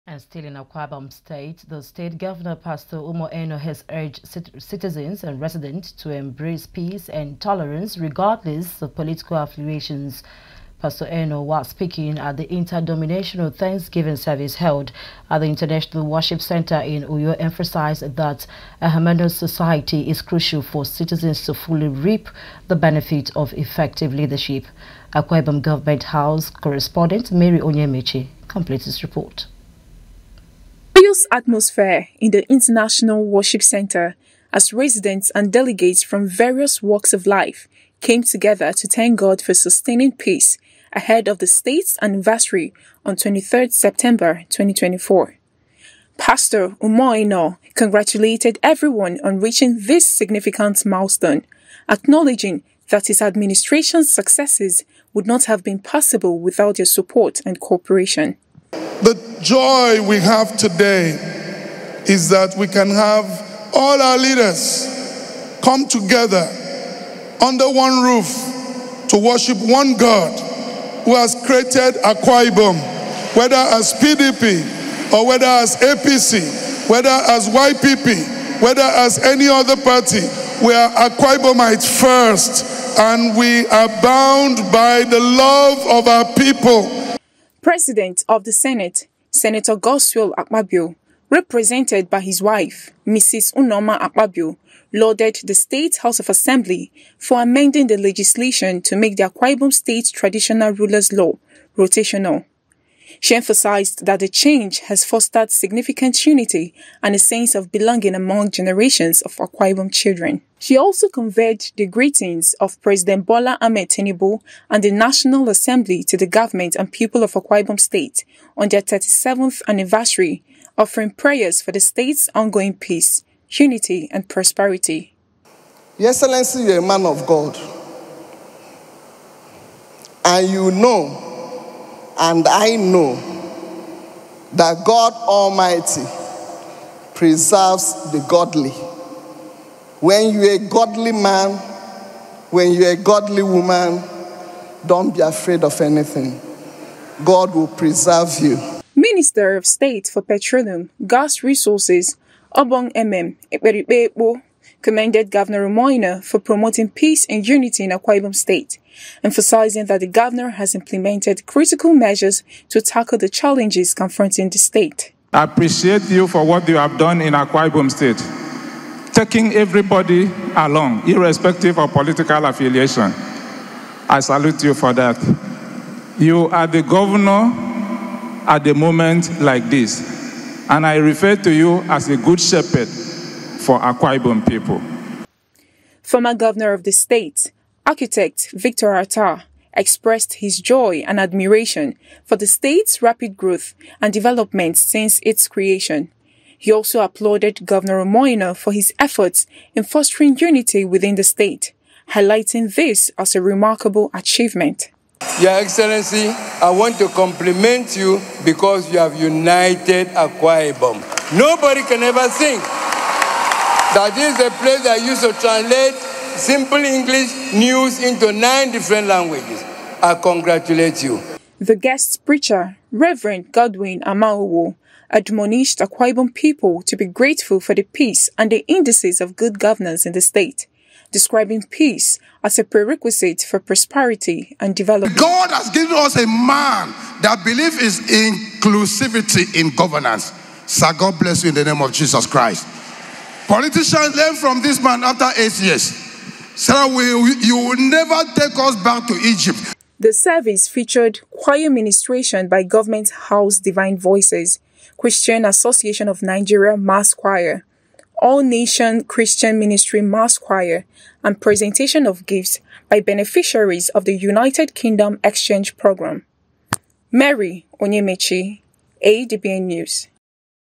AKWA IBOM ANNIVERSARY PASTOR ENO URGES PEACE
Speaking at an inter-denominational thanksgiving service held at the International Worship Centre in Uyo, Pastor Eno emphasized that a harmonious society is essential for citizens to fully enjoy the benefits of effective leadership. He urged the people of Akwa Ibom to unite, highlighting the importance of cooperation in fostering progress and development within the state.

 Mary Onyemaechi, ADBN News.